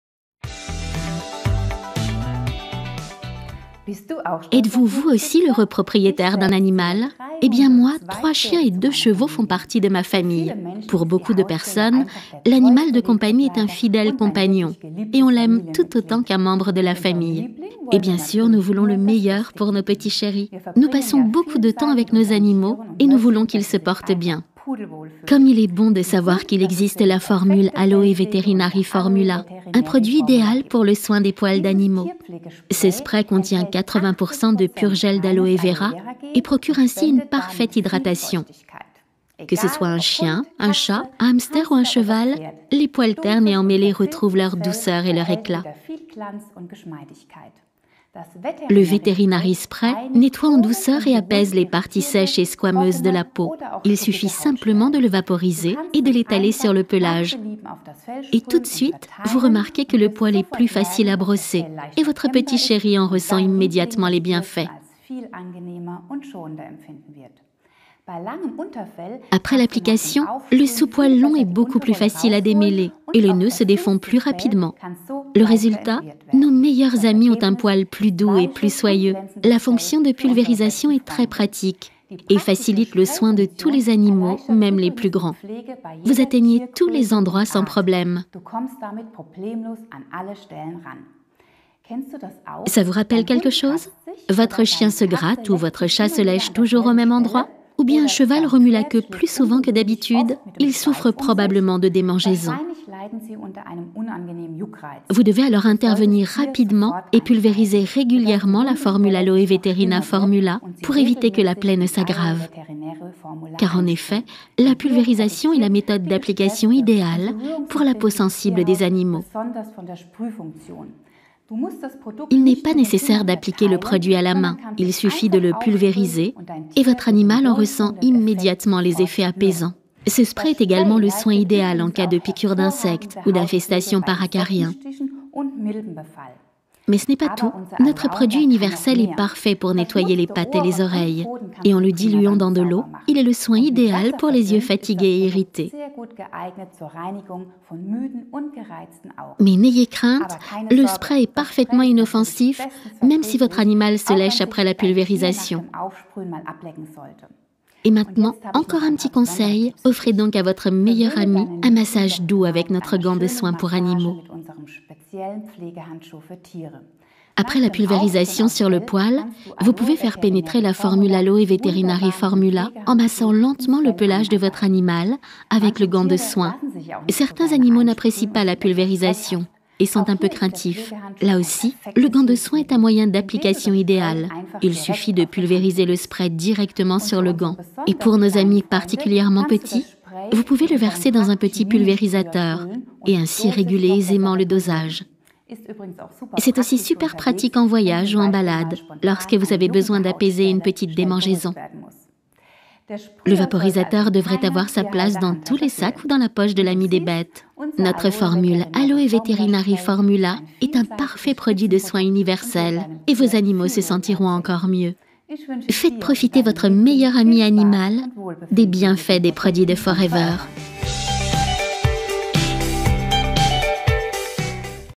Forever Formule Animale Aloès / Aloe Veterinary Formula (Ref. 30)
Heureusement qu’il existe le spray hydratant Aloe Veterinary Formula pour le soin parfait du poil. Il contient le 80% de pure Aloe Vera qui hydrate la peau. Le poil terne et cassant retrouve son éclat et sa souplesse. Il regroupe plusieurs indications en un seul produit : soin du pelage, lotion d’hygiène, calme les démangeaisons, favorise la cicatrisation.